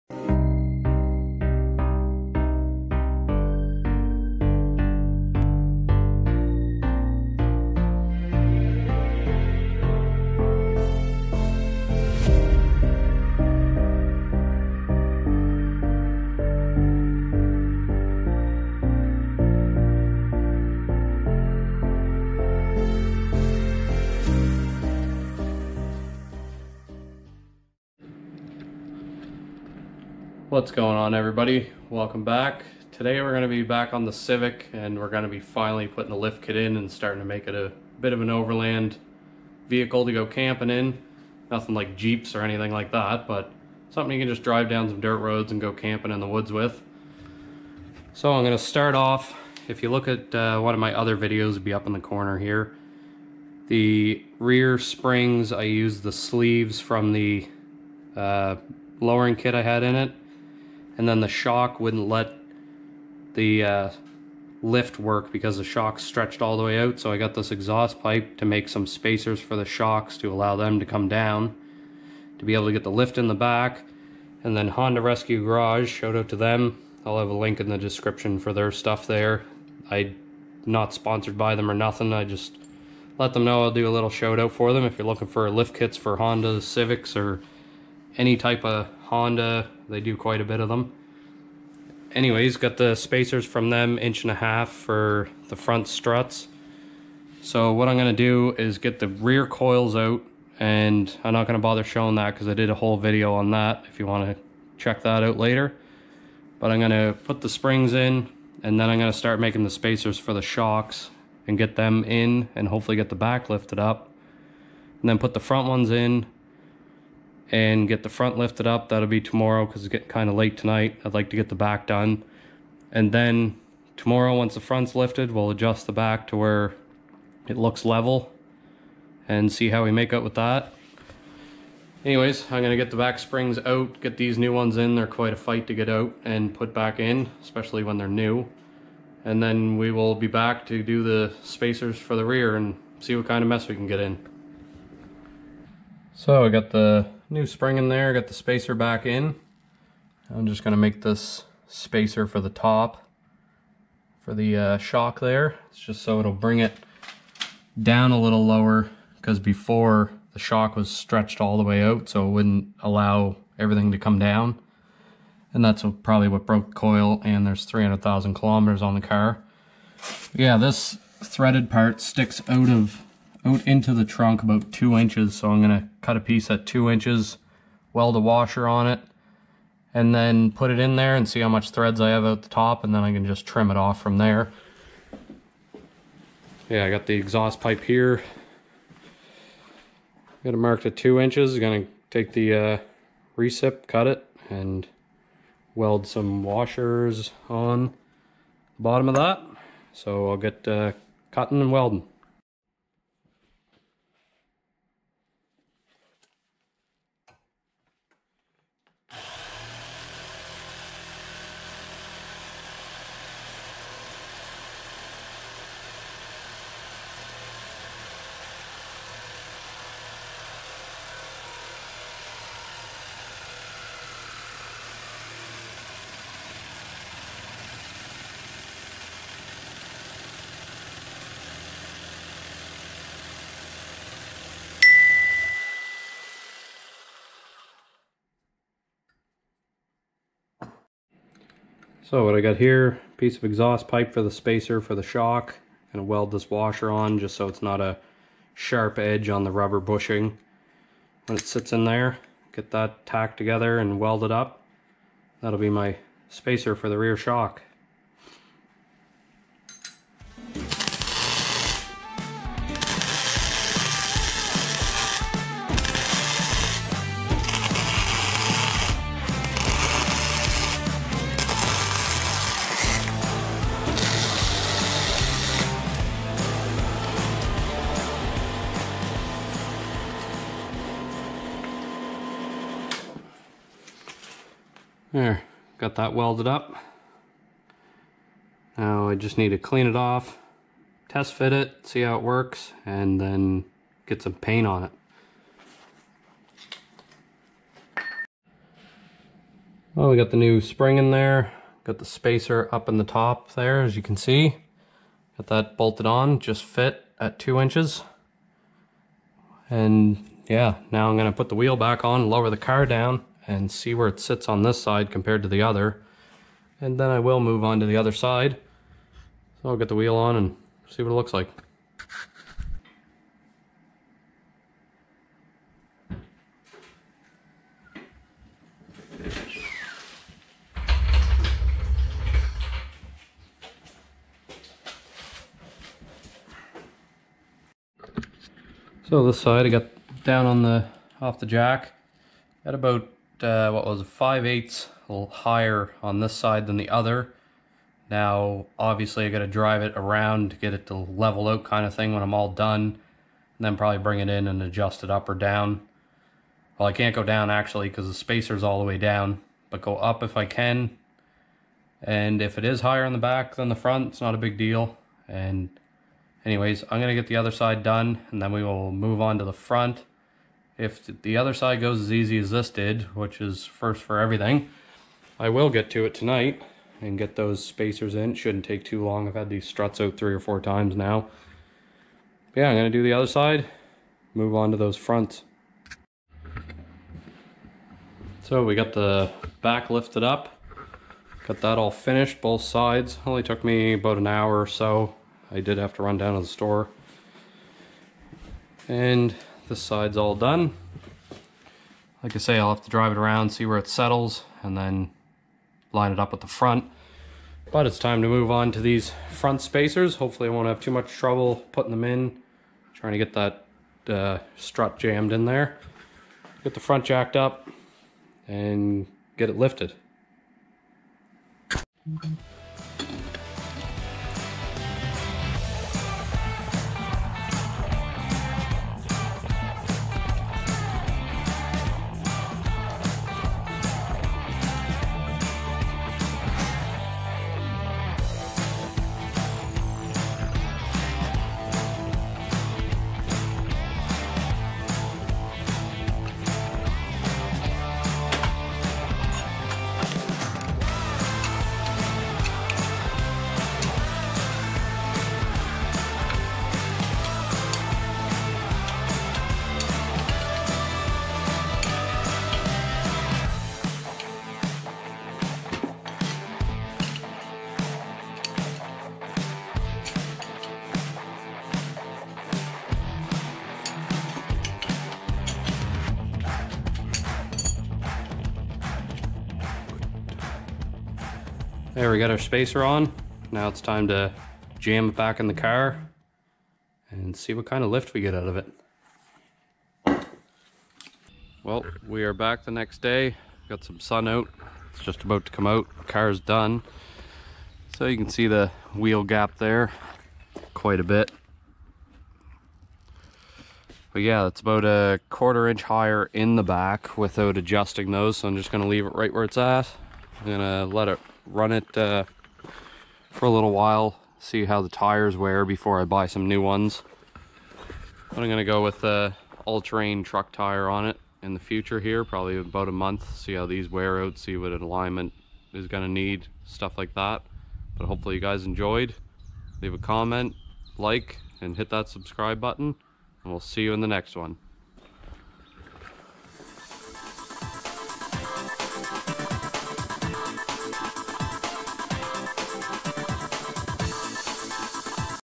Honda civic lift kit installed!
hey welcome back everyone today I will be installing the lift kit on my 8th gen Honda civic with some spacers in the front from Honda rescue garage and some spacers I made for the back hope you enjoy and don't for get to check out hrg website for your Honda lift needs!!